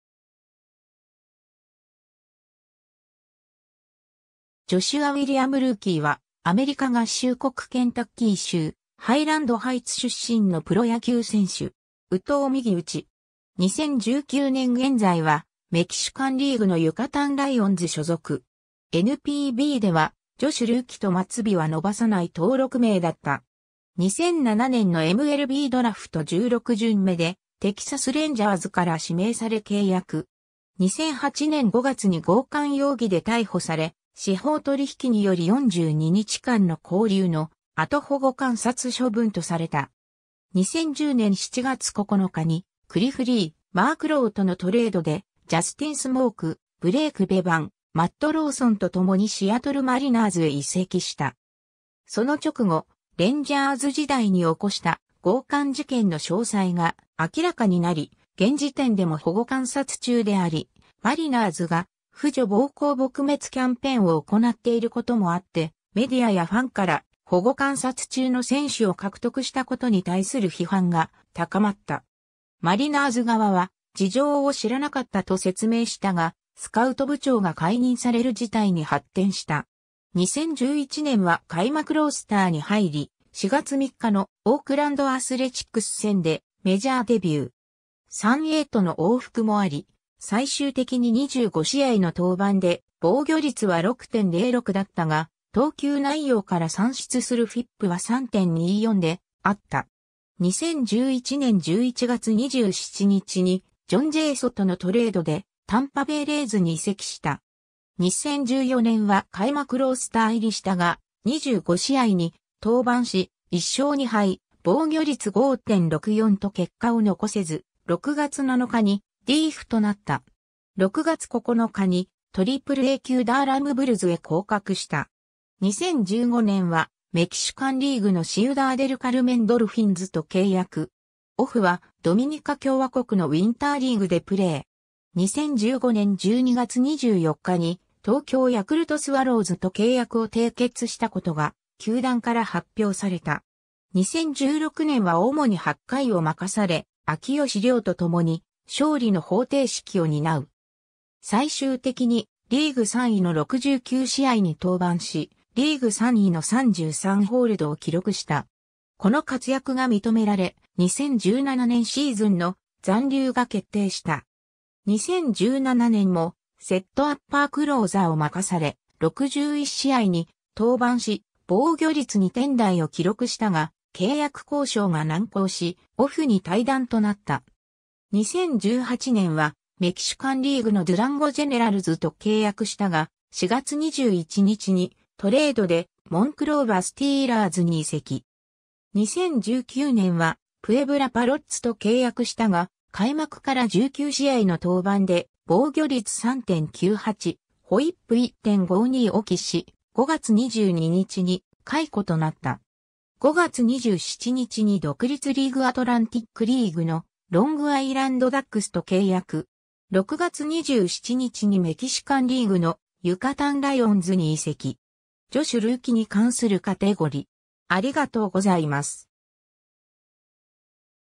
ジョシュ・ルーキー
シウダーデルカルメン・ドルフィンズの選手
ドゥランゴ・ジェネラルズの選手
モンクローバ・スティーラーズの選手
ジョシュア・ウィリアム・ルーキー（Joshua William Lueke、1984年12月5日 - ）は、アメリカ合衆国ケンタッキー州ハイランドハイツ出身のプロ野球選手（投手）。
右投右打。
2019年現在は、メキシカンリーグのユカタン・ライオンズ所属。
 NPBでは「ジョシュ・ルーキ」と末尾は伸ばさない登録名だった。
 2007年のMLBドラフト16巡目（全体500位）でテキサス・レンジャーズから指名され契約。
 2008年5月に強姦容疑で逮捕され、司法取引により42日間の勾留の後保護観察処分とされた。
 2010年7月9日にクリフ・リー、マーク・ロウとのトレードで、ジャスティン・スモーク、ブレイク・ベバン、マット・ローソンと共にシアトル・マリナーズへ移籍した。
その直後、レンジャーズ時代に起こした強姦事件の詳細が明らかになり、現時点でも保護観察中であり、マリナーズが婦女暴行撲滅キャンペーンを行っていることもあって、メディアやファンから保護観察中の選手を獲得したことに対する批判が高まった。
マリナーズ側は「事情を知らなかった」と説明したが、スカウト部長が解任される事態に発展した。
 2011年は開幕ロースターに入り、4月3日のオークランド・アスレチックス戦でメジャーデビュー。
3Aとの往復もあり、最終的に25試合の登板で防御率は6.06だったが、投球内容から算出するFIPは3.24であった。
 2011年11月27日にジョン・ジェイソとのトレードでタンパベイ・レイズに移籍した。
 2014年は開幕ロースター入りしたが、25試合に登板し、1勝2敗、防御率5.64と結果を残せず、6月7日にDFAとなった。
6月9日にAAA級ダーラム・ブルズへ降格した。
 2015年はメキシカンリーグのシウダーデルカルメン・ドルフィンズと契約。
オフはドミニカ共和国のウィンターリーグでプレー。
 2015年12月24日に、東京ヤクルトスワローズと契約を締結したことが球団から発表された。
 2016年は主に8回を任され、秋吉亮と共に勝利の方程式を担う。
最終的にリーグ3位の69試合に登板し、リーグ3位の33ホールドを記録した。
この活躍が認められ、2017年シーズンの残留が決定した。
 2017年もセットアッパー・クローザーを任され、61試合に登板し防御率2点台を記録したが、契約交渉が難航し、オフに退団となった。
 2018年はメキシカンリーグのドゥランゴ・ジェネラルズと契約したが、4月21日にトレードでモンクローバ・スティーラーズに移籍。
 2019年はプエブラ・パロッツと契約したが、開幕から19試合の登板で防御率3.98、WHIP1.52を喫し、5月22日に解雇となった。
5月27日に独立リーグ・アトランティックリーグのロングアイランド・ダックスと契約。
6月27日にメキシカンリーグのユカタン・ライオンズに移籍。
 ジョシュ・ルーキに関するカテゴリ: